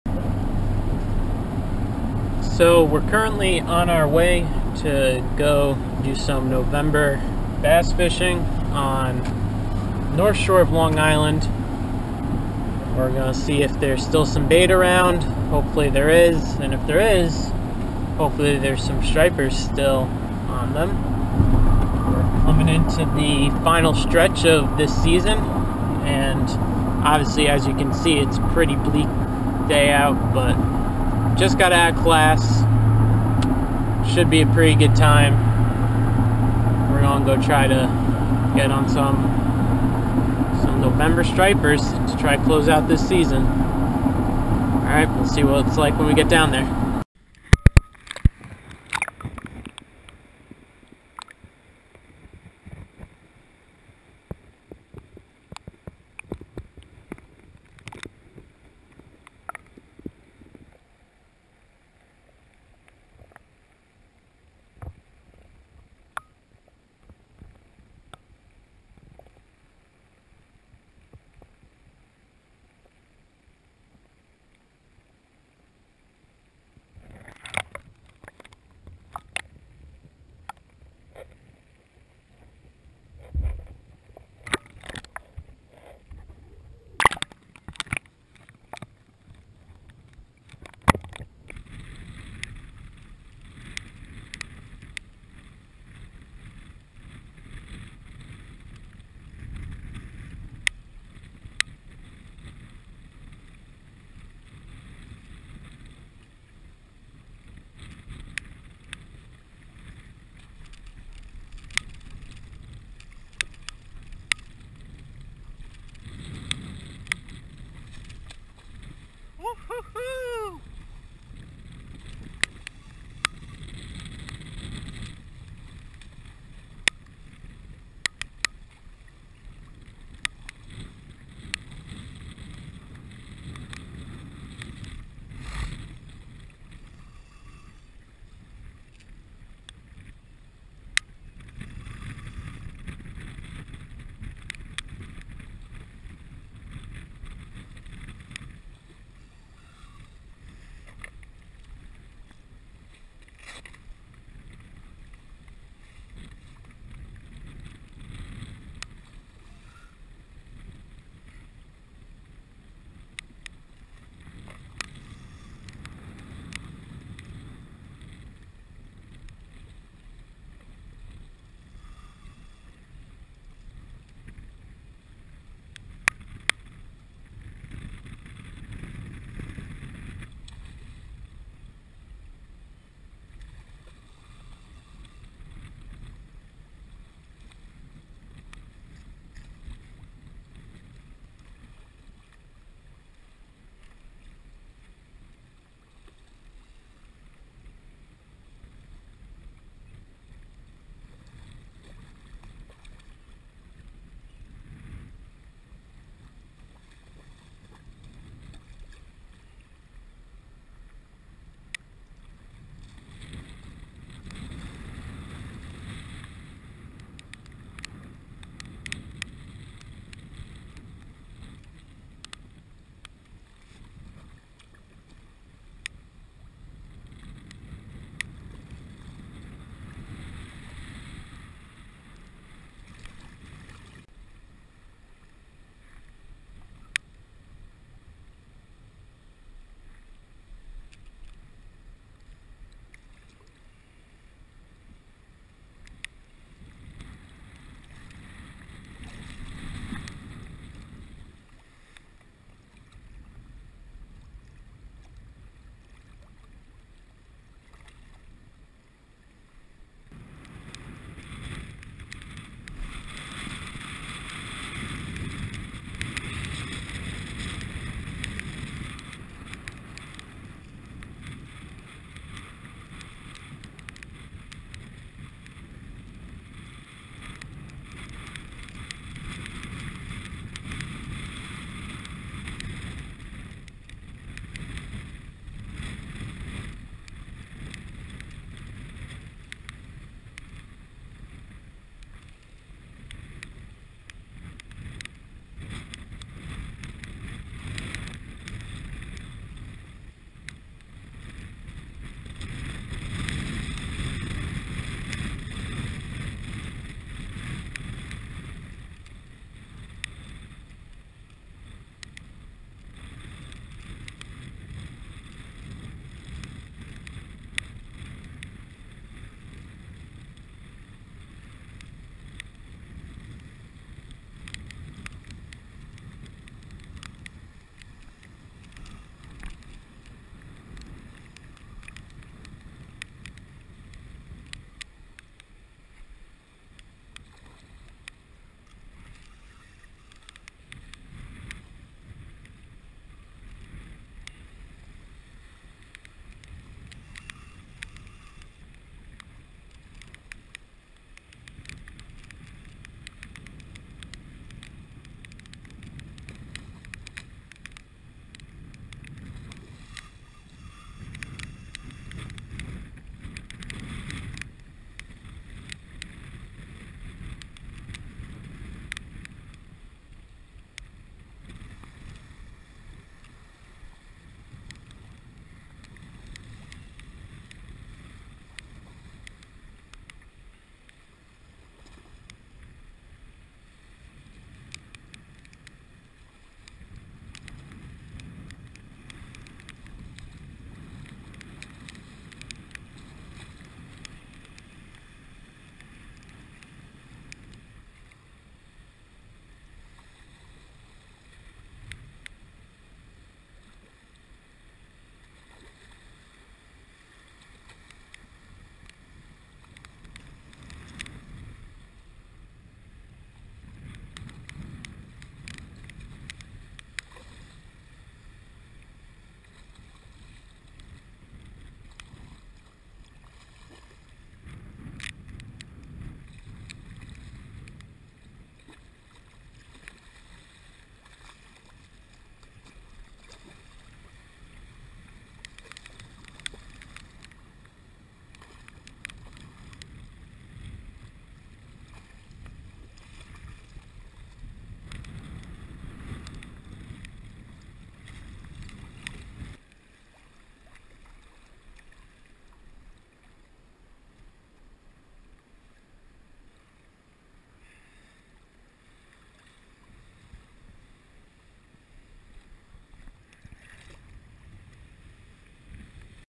Insane Topwater Gorilla Bluefish
Here is a video of fishing some foul November weather fishing.